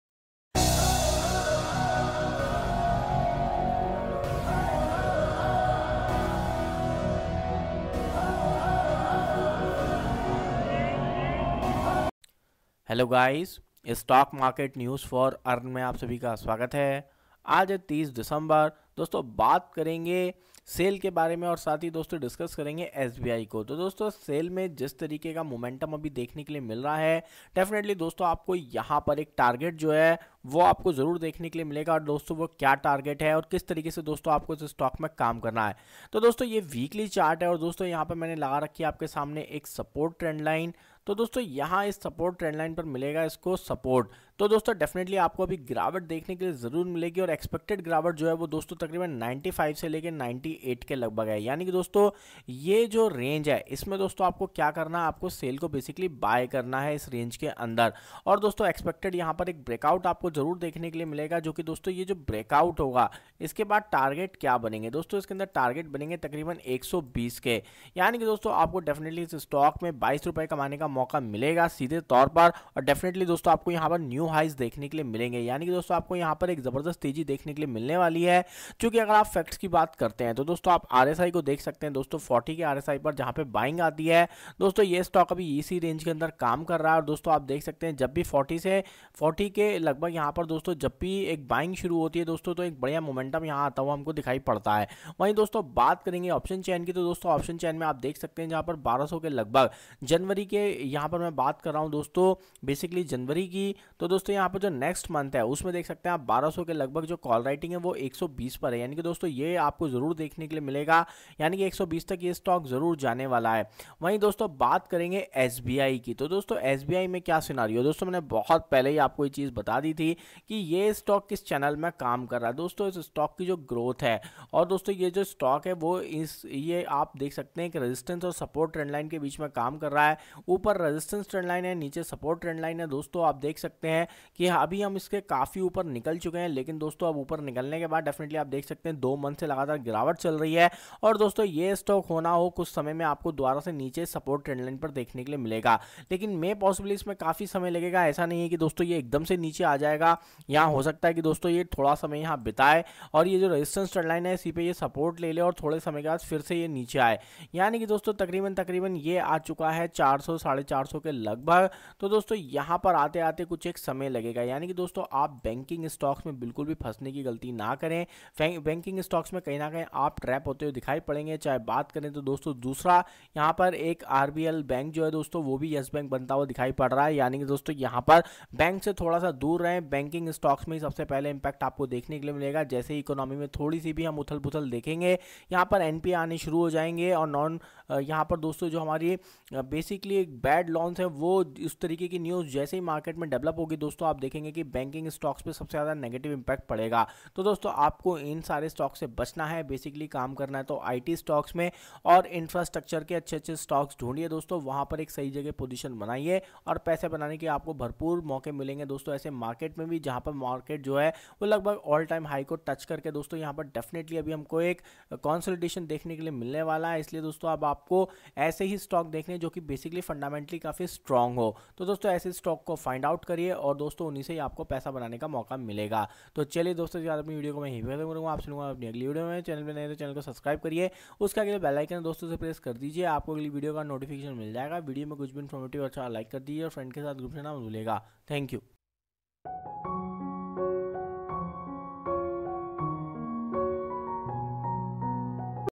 SAIL इस Range Buy करो में 120 का Target जल्द मिलेगा, Banking Stocks से दूर रहो / Stock News /
In this video, I analyzed SAIL & SBI on the basis of quantitative and technical analysis.